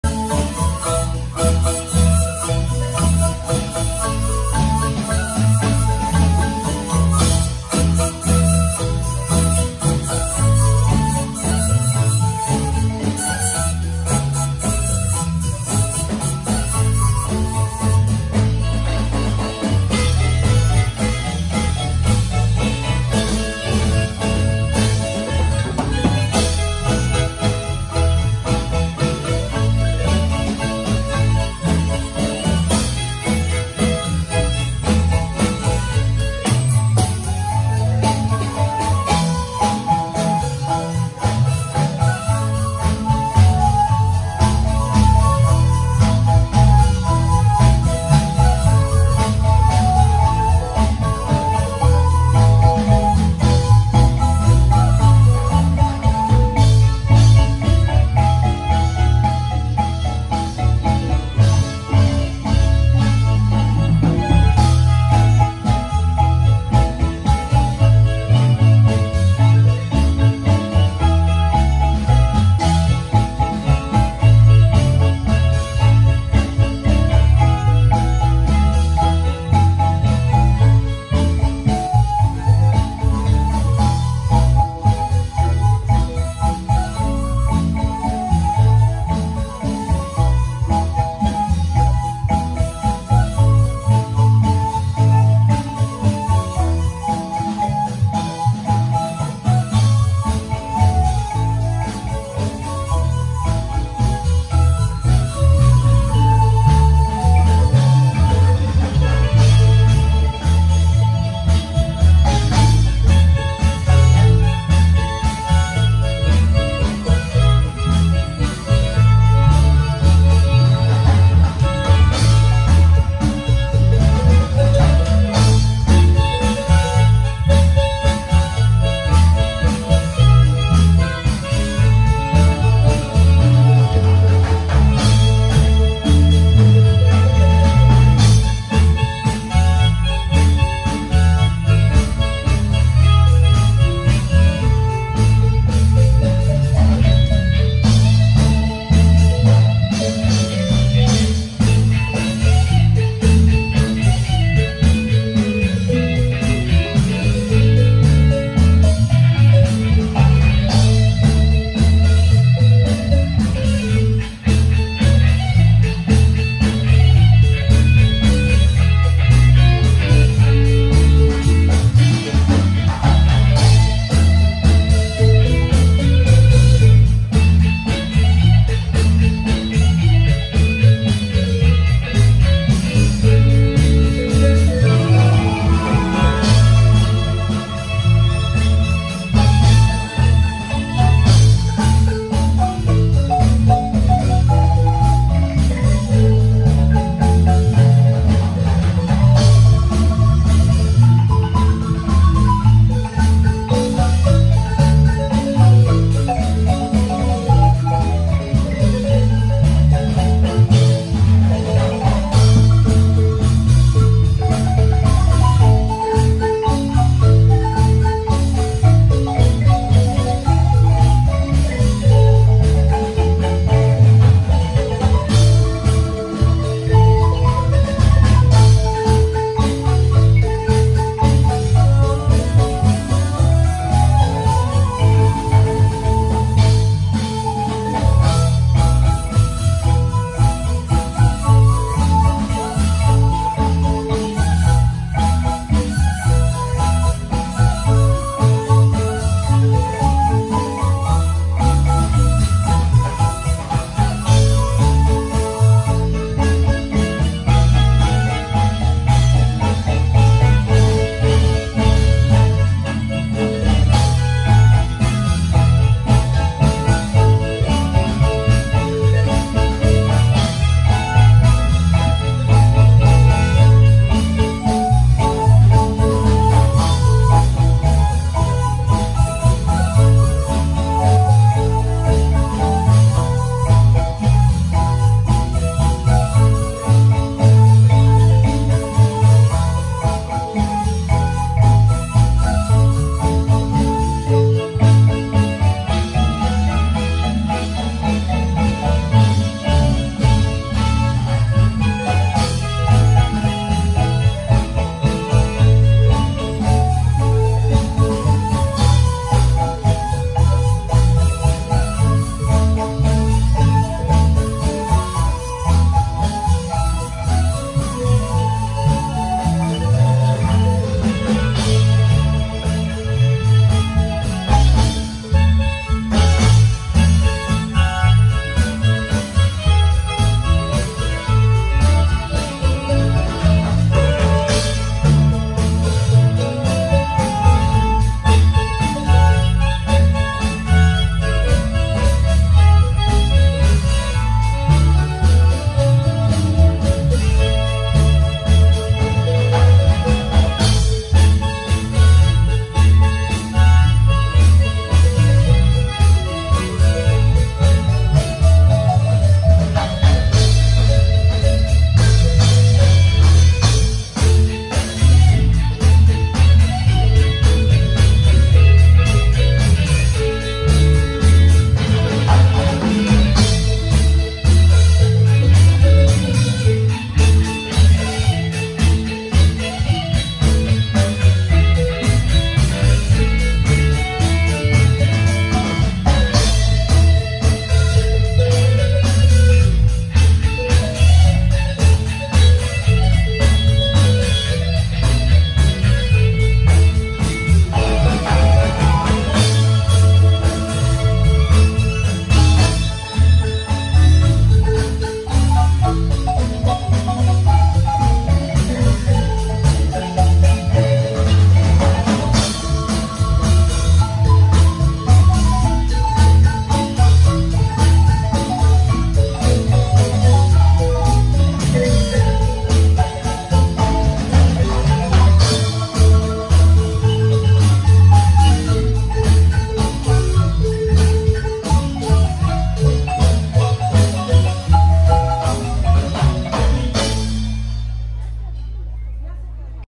Sonal Kokonob’ USA-Perris California 2020.Boda de Tony y Lorenza.Video#28
Sonal Kokonob’ USA Desde Perris,California.22/8/2020.